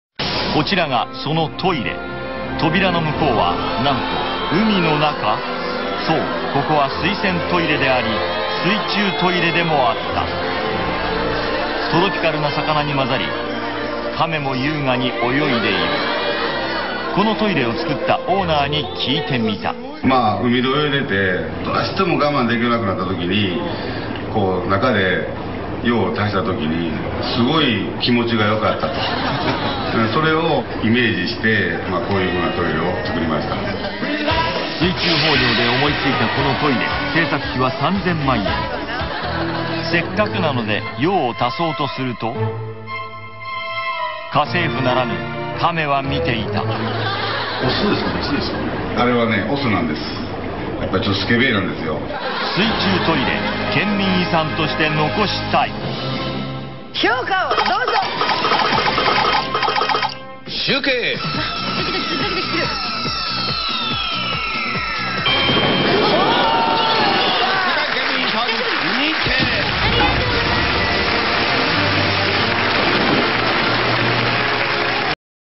wc aquarium
Présentation de WC au milieu d'un aquarium géant .